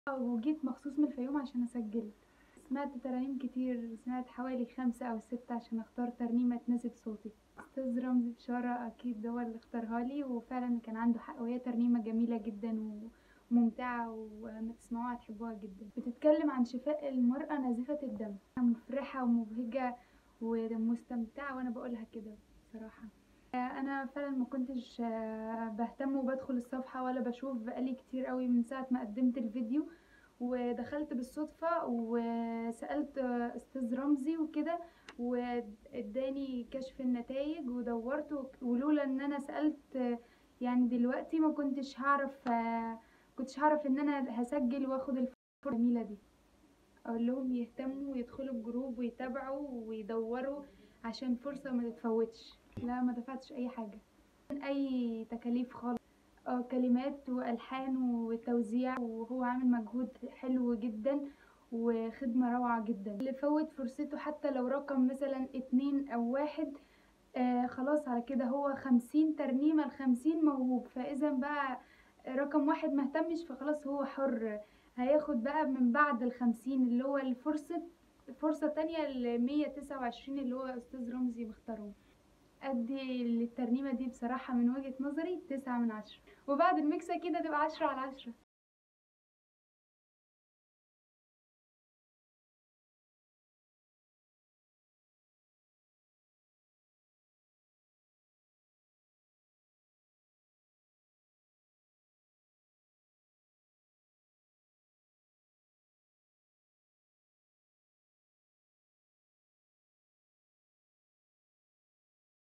ماريا مجدي: ترنيمتي في مشروع الخمسين موهوب تاخد 9 على 10 فقط
مشروع يهدف لإنتاج 50 ترنيمة لخمسين موهوب بدون أي مقابل في أي مرحلة